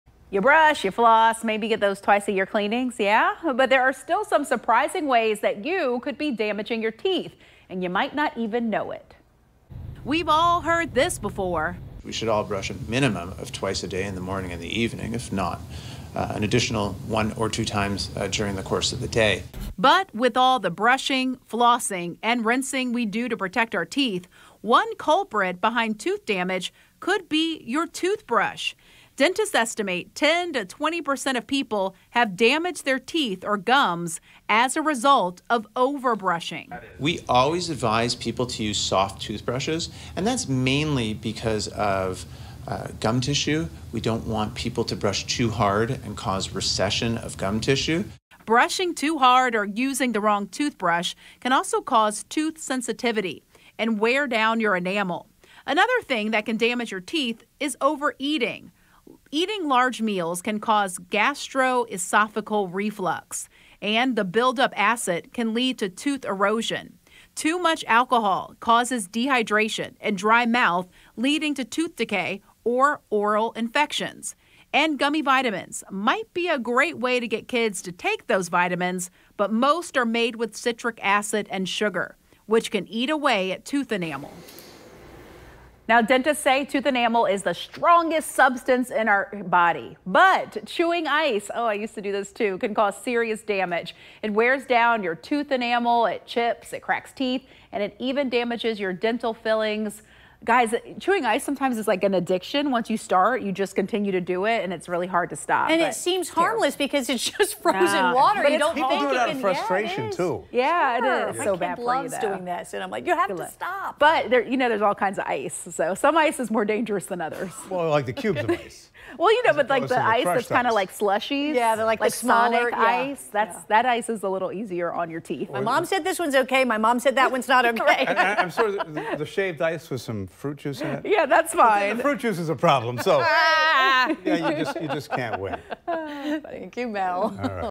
Hurting Your Teeth
You may brush, floss an get your bi-yearly teeth cleanings, but you could still be damaging your teeth without even realizing it.